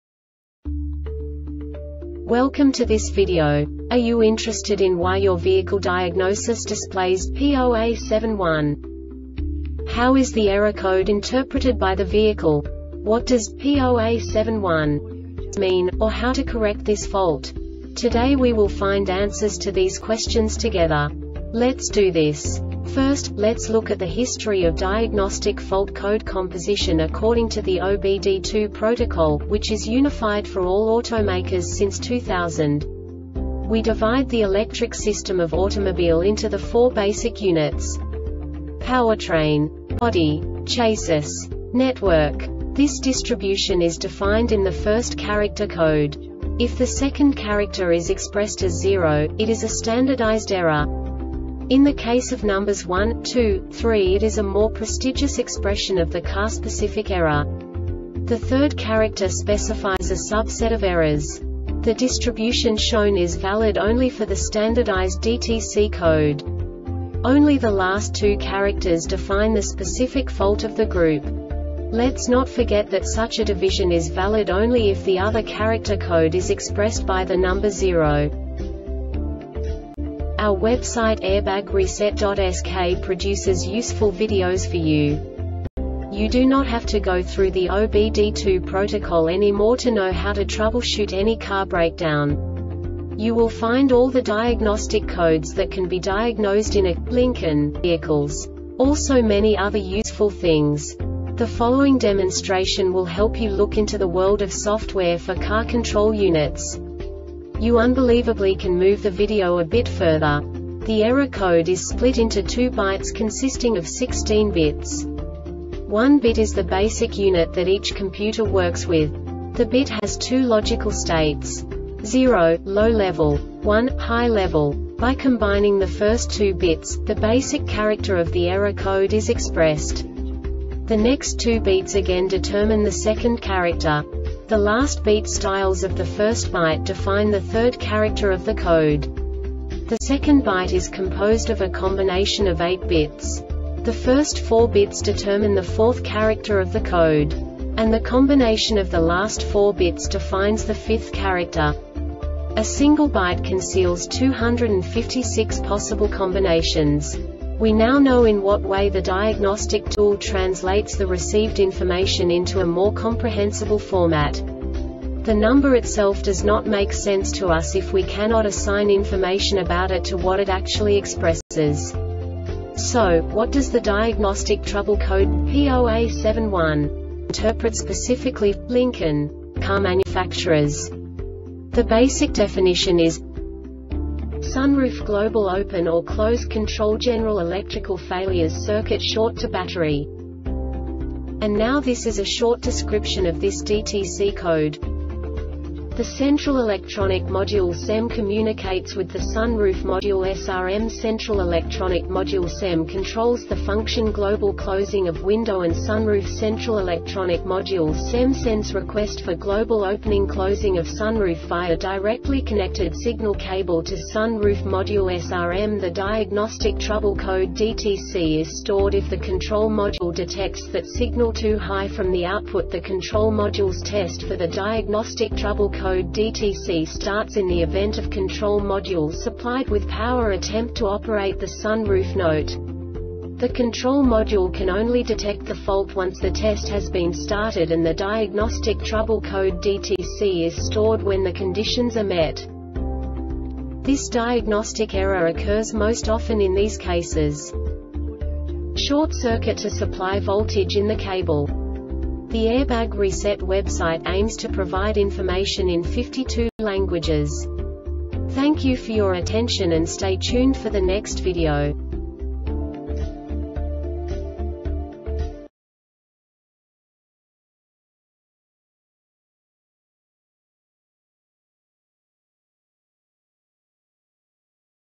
DTC Lincoln P0A71 Short Explanation
Contents:
0:21 Basic DTC analysis according to OBD2 protocol standard.
1:48 Insight into programming
2:58 A diagnostically understandable description of the Diagnostic Trouble Code
3:05 Basic error definition

Make:
Lincoln
Code:
P0A71
Definition:
Sunroof Global Open or Close Control. General Electrical Failures. Circuit Short To Battery
Description:
The central electronic module (CEM) communicates with the sun roof module (SRM).Central electronic module (CEM) controls the function globalorclosing of window and sunroof.Central electronic module (CEM) sends request for global opening or closing of sunroof via directly connected signal cable to Sun roof module (SRM).The diagnostic trouble code (DTC) is stored if the control module detects that: Signal too high from the output.The control modules test for the diagnostic trouble code (DTC) starts in the event of: Control module supplied with power.Attempt to operate the sun roof.Note! The control module can only detect the fault once the test has been started and the diagnostic trouble code (DTC) is stored when the conditions are met.
Cause:
Short-circuit to supply voltage in the cable.